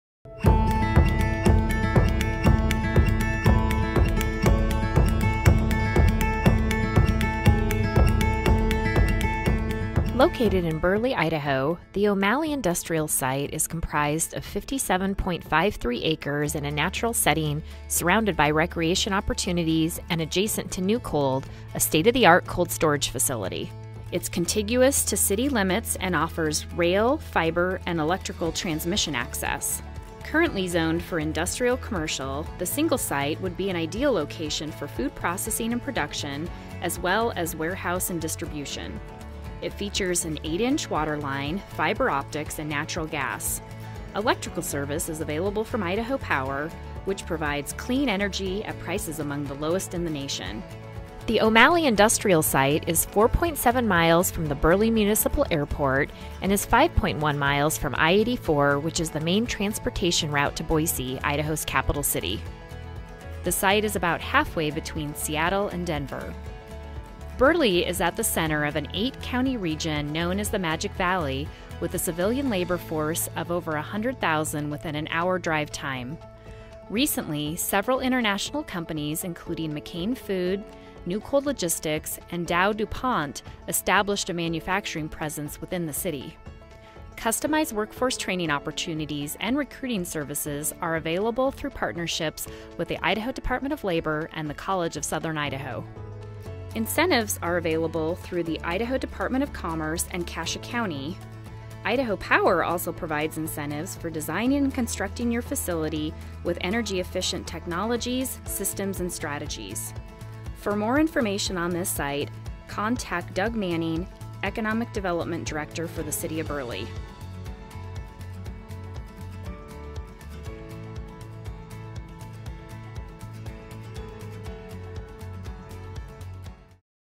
O’Malley Industrial Site, Burley, Idaho
Zoned for commercial industrial, this 57.53-acre site is ready to meet your energy needs.